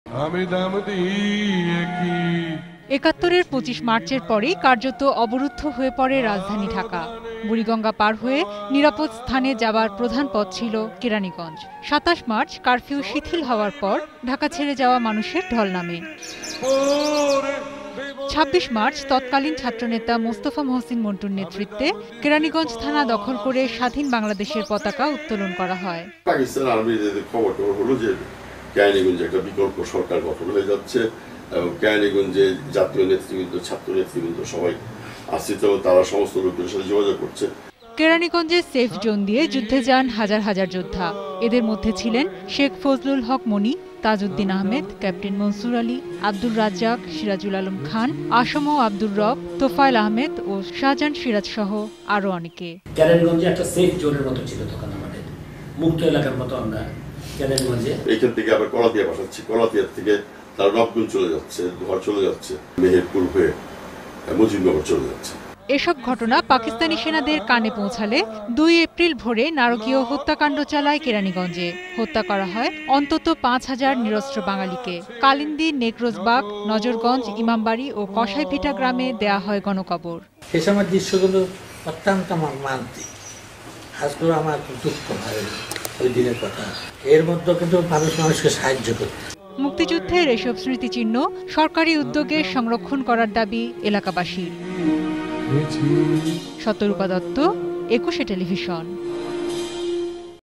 মুক্তিযুদ্ধের কালের সাক্ষী কেরাণীগঞ্জ
| ETV News

রাজধানীর উপকণ্ঠ কেরাণীগঞ্জ মুক্তিযুদ্ধের কালের সাক্ষী। সাতই মার্চের ভাষণের পর বঙ্গবন্ধুর নির্দেশে মুক্তিযুদ্ধের প্রস্তুতি শুরু হলে, কেরানীগঞ্জেও শুরু হয় অস্ত্র প্রশিক্ষণ।  ২৫ মার্চ ঢাকায় গণহত্যার পর স্বাধীকার আন্দোলনের অনেক নেতা অবস্থান নেন  কেরানীগঞ্জে ।
একাত্তরের ২৫ মার্চের পরই কার্যত অবরুদ্ধ হয়ে পড়ে রাজধানী ঢাকা। বুড়িগঙ্গা পার হয়ে নিরাপদ স্থানে যাবার প্রধান পথ ছিলো কেরানীগঞ্জ। ২৭ মার্চ কারফিউ শিথিল হওয়ার পর ঢাকা ছেড়ে যাওয়া মানুষের ঢল নামে ।
২৬  মার্চ তৎকালীন ছাত্রনেতা মোস্তাফা মোহসীন মন্টুর নেতৃত্বে কেরানীগঞ্জ থানা দখল করে স্বাধীন বাংলাদেশের পতাকা উত্তোলন করা হয়।
কেরানীগঞ্জের সেইফ জোন দিয়ে যুদ্ধে যান হাজার হাজার যোদ্ধা।
এসব ঘটনা পাকিস্তানী সেনাদের কানে পৌঁছালে ২ এপ্রিল ভোরে নারকীয় হত্যাকান্ড চালায় কেরানীগঞ্জে। হত্যা করায় হয় অন্তত পাঁচ হাজার নিরস্ত্র বাঙালীকে। কালিন্দি, নেকরোজবাগ, নজরগঞ্জ, ইমামবাড়ি ও কসাইভিটা গ্রামে দেয়া হয় গণকবর।
মুক্তিযুদ্ধেও এসব স্মৃতিচিহ্ন সরকারি উদ্যোগে সংরক্ষণ করার দাবি এলাকাবাসীর।